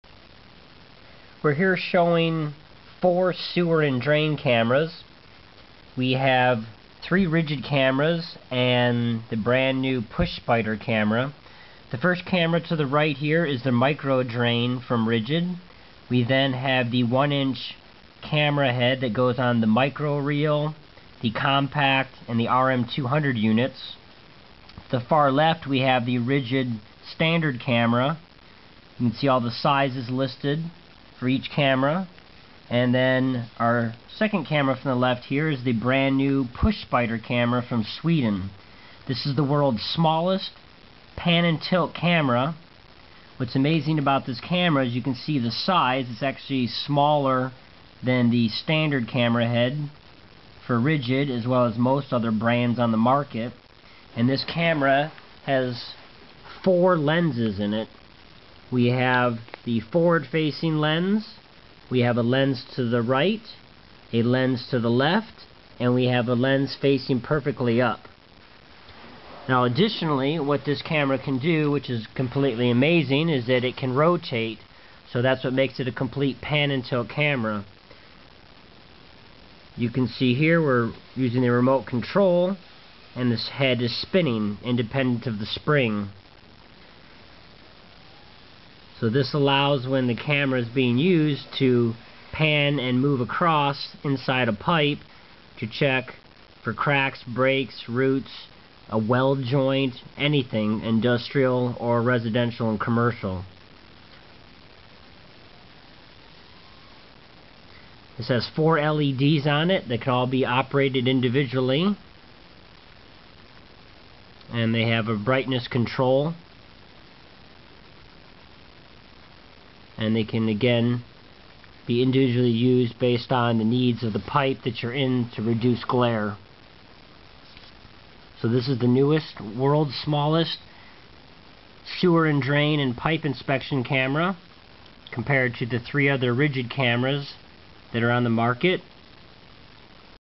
World's smallest pan & tilt drain, pipe, and sewer camera
Push Spider Camera from 78th of Sweden compared to Ridgid SeeSnake cameras